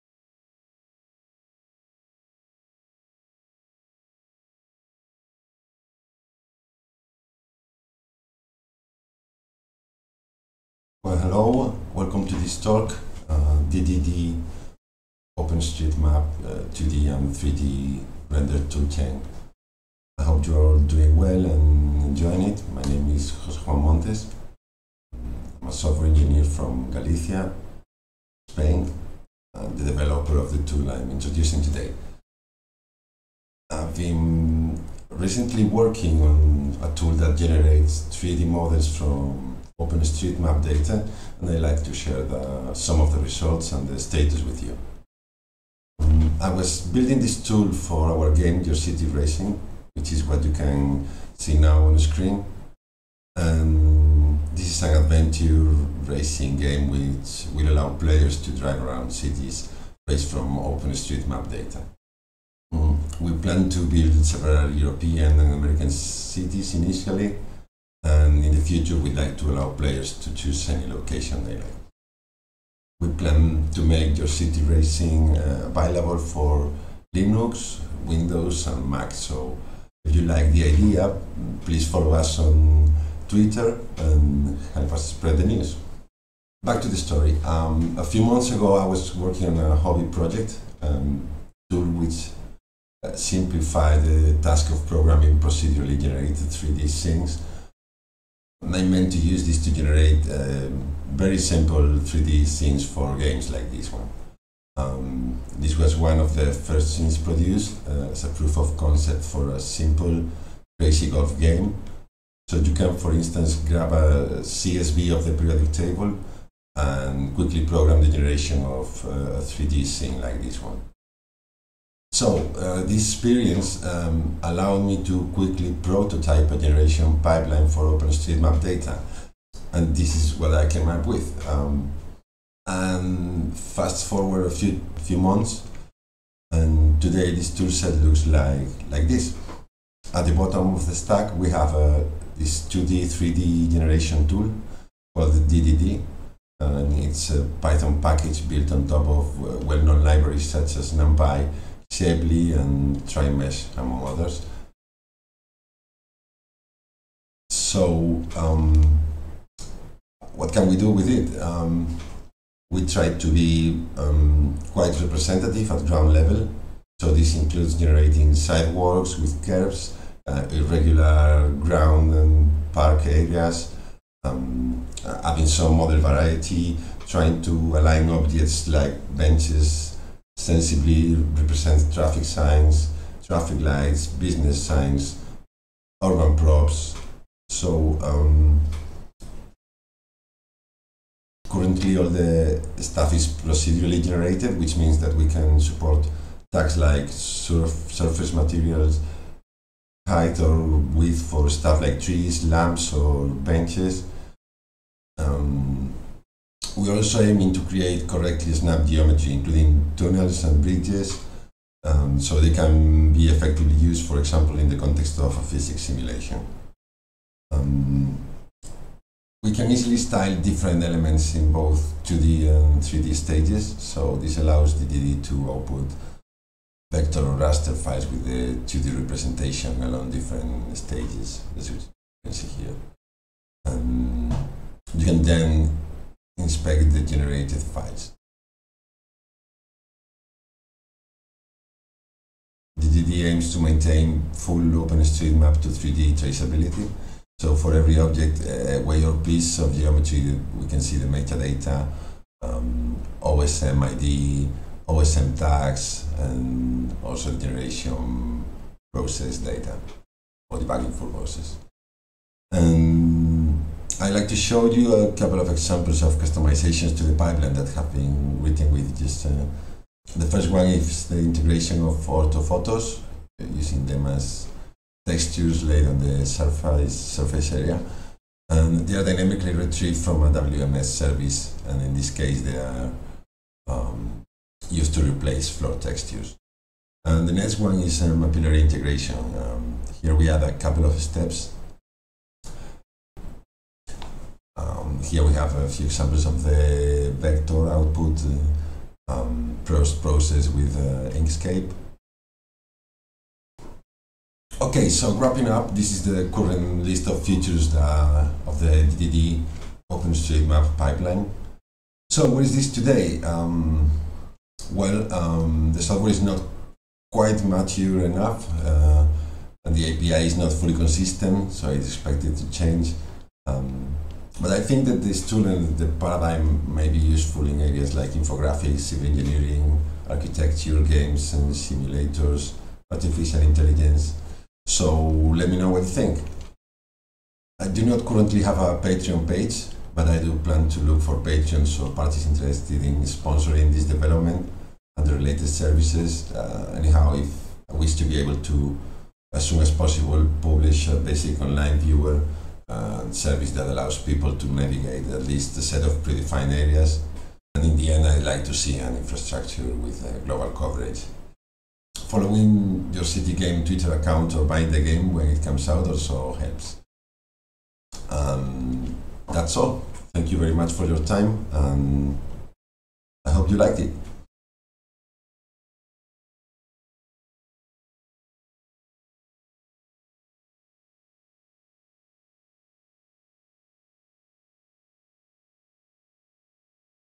Generating 3D from OpenStreetMap data (Lighting Talk)
Lighting talk introducing a 2D/3D procedural generation pipeline to generate 3D models from OpenStreetMap data.

I plan to release this toolchain as open source on GitHub after cleaning up the codebase and adding some documentation. Do not hesitate to get in touch if you are interested.

Since this talk was published, I published a web under (check the About page to understand how to use it, and some pre-generated areas).

I'm also currently using this tool to help me create some maps for my arcade game: "Rope Cow", have a look!

This lighting talk was broadcasted on the "OSM State of the Map" conference You can also find more related videos in this channel.